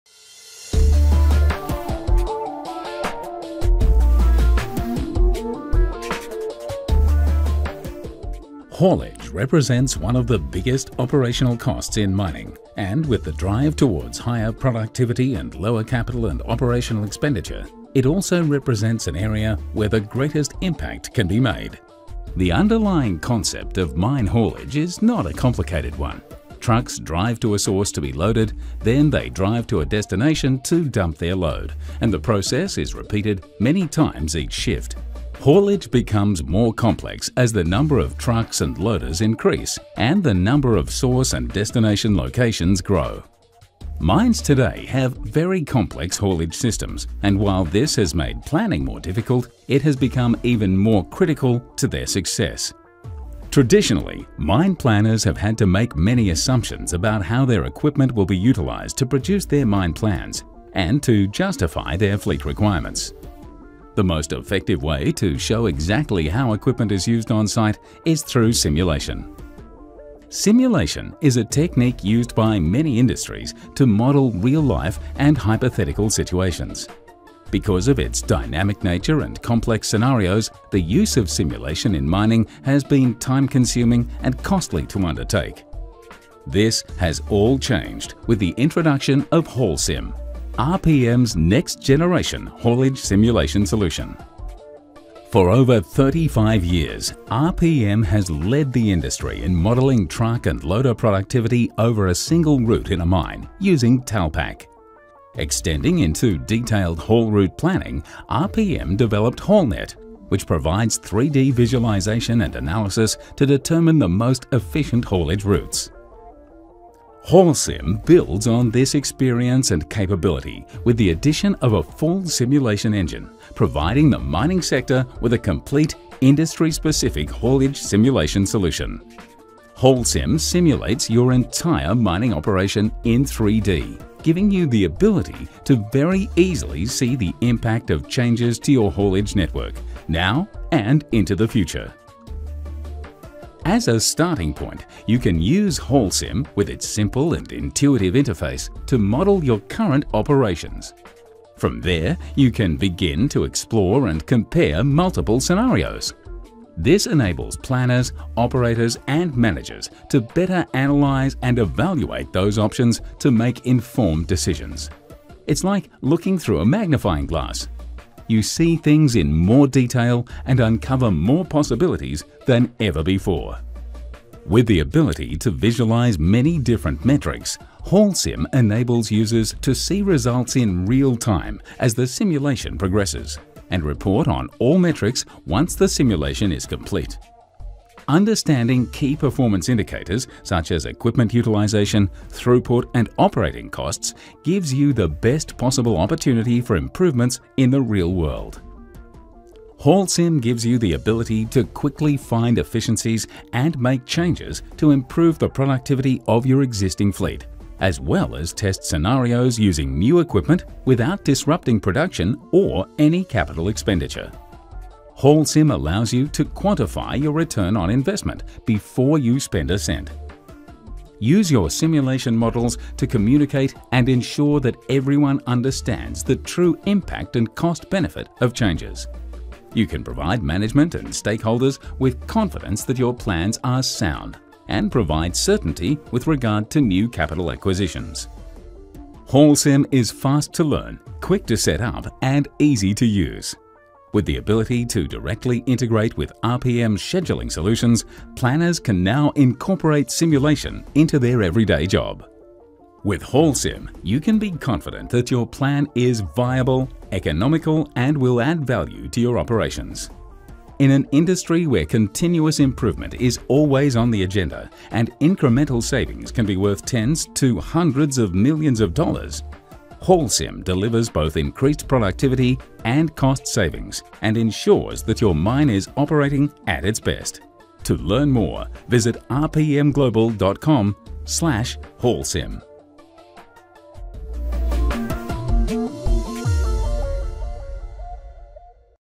Видео о пакете HAULSIM
Ознакомительный видеоролик о решении для имитационного моделирования процессов откатки -- пакете HAULSIM. С помощью пакета Вы сможете создать точную модель работы горнотранспортной системы, быстро опробовать множественные сценарии работы и принять решение на основе полученных данных.